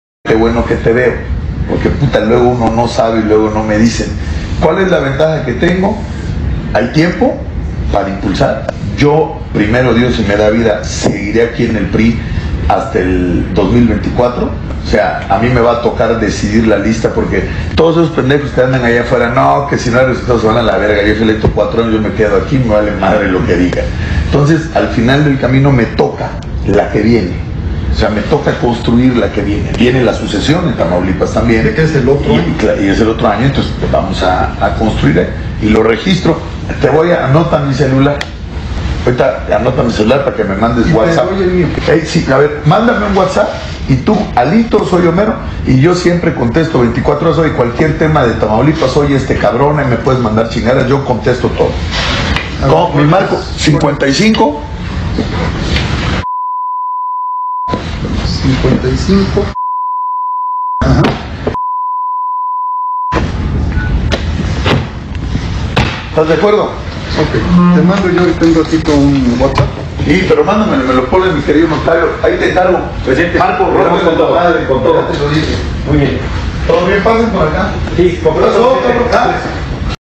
Se burla Alejandro Moreno Cárdenas de los priistas ¡Ya se quitó la máscara! Asegura Layda Sansores
Se burla de los priistas... ¡Ya se quitó la máscara!
Si a la gente de su mismo partido la engaña y le roba, ¡qué puede esperar el pueblo campechano y todo el pueblo mexicano de una persona cínica y que no tiene vergüenza!